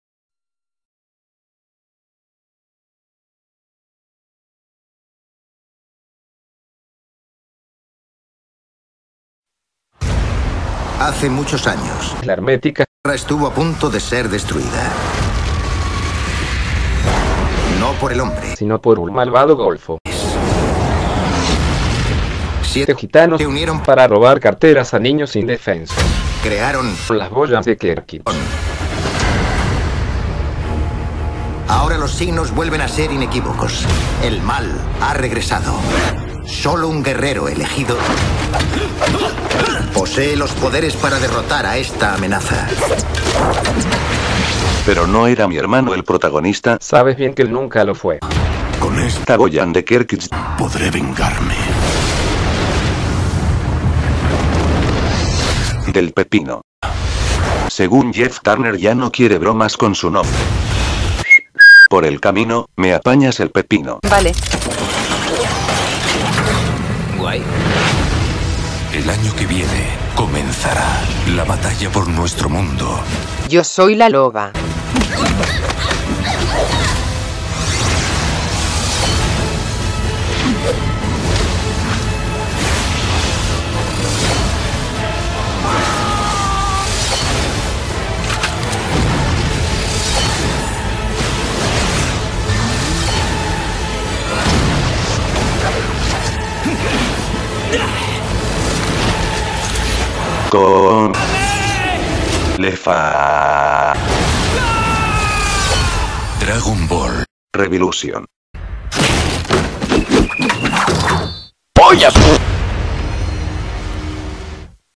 DRAGON BALL: REVILLUTION TRAILER
La Loba Productions tiene el placer de presentar su nueva producción de loba para gitanos. En esta ocasión, la historia de la Selva Hermética continua, pero con la ausencia de su hipotético protagonista, ya que ahora su vida se enfoca a otros menesteres de la vida adulta, lo cual le priva de estas bromas. De todos modos, la vida sigue adelante y las historias siempre deben cambiar de protagonista. Y quien hay mejor que el Coronel Gargamelio para ser un protagonista a la altura!!! Adelante, Gargamelio con tu rabo pepinator para vencer el mal de Sagalás!!